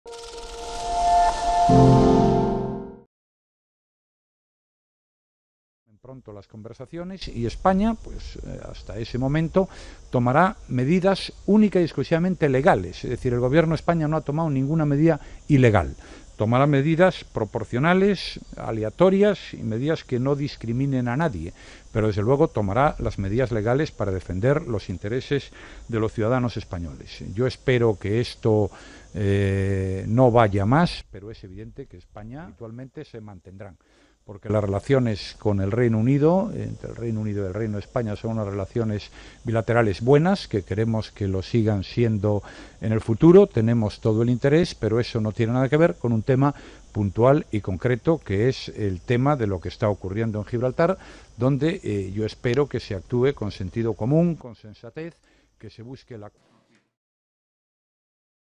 Spain's prime minister Mariano Rajoy calls for 'common sense' over Gibraltar
The Spanish prime minister, Mariano Rajoy, has promised to engage Britain in dialogue over the issue of Gibraltar, but says that he will defend his country's interests. After a week of rising tensions, he said he hoped 'common sense' would prevail in the dispute